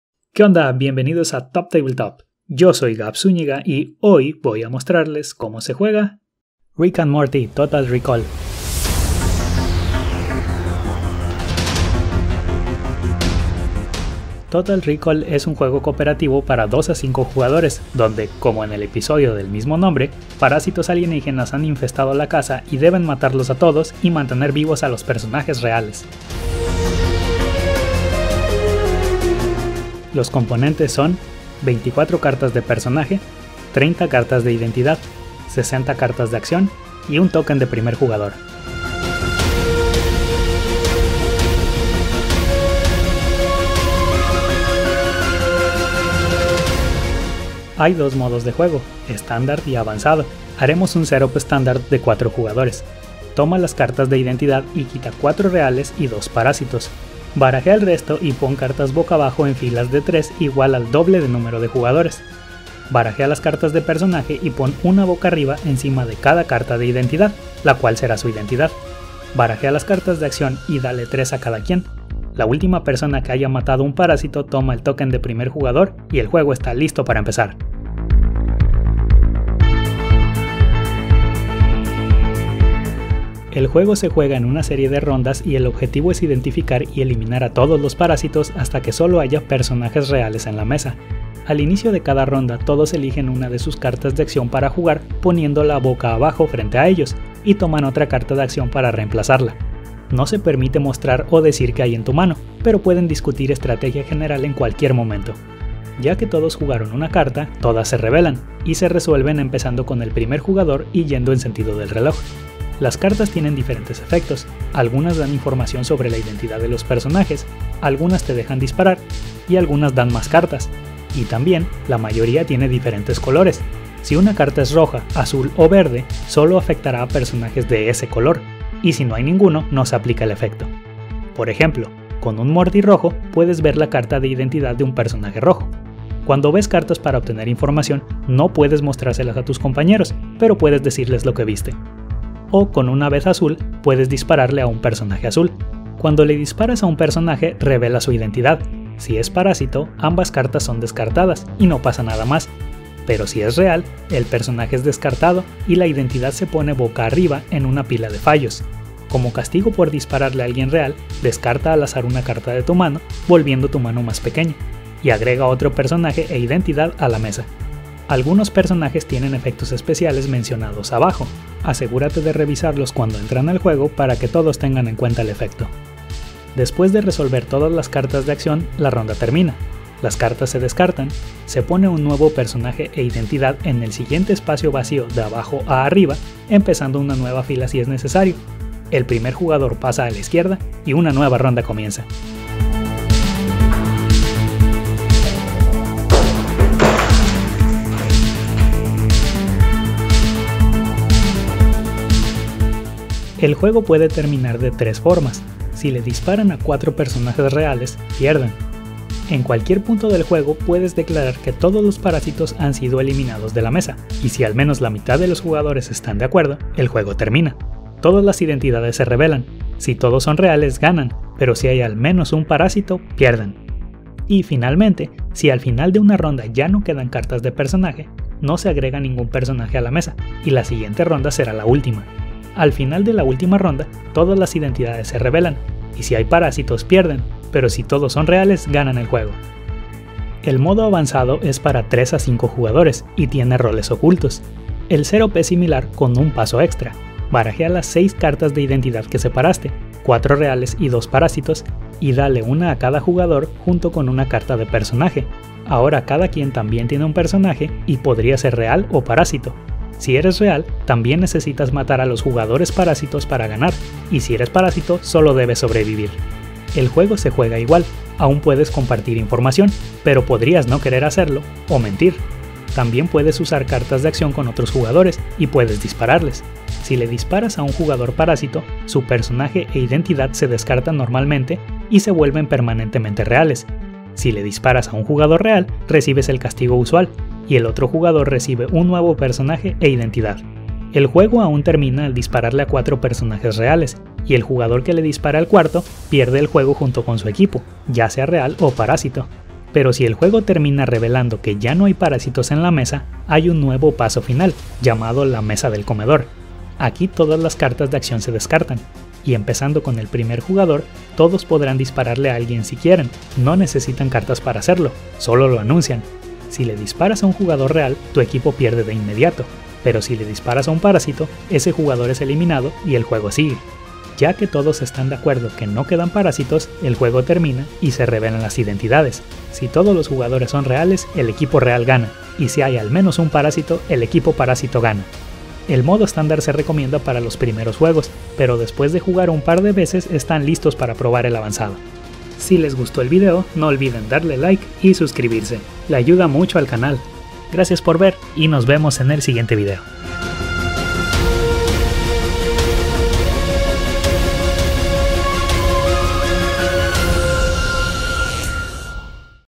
Rick and Morty Total Rickall - Como Jugar [Elimina los parásitos!]
Aprende como se juega Rick and Morty Total Rickall y elimina la infestación de parásitos alienígenas!

Gracias por sus likes y suscripciones, ayudan mucho al canal!

Checa el canal!

00:00 Intro
00:26 Componentes
00:39 Setup
01:18 Como jugar
01:52 Cartas de acción
02:25 Disparar
02:56 Fin de la ronda
03:12 Fin del juego
04:00 Modo avanzado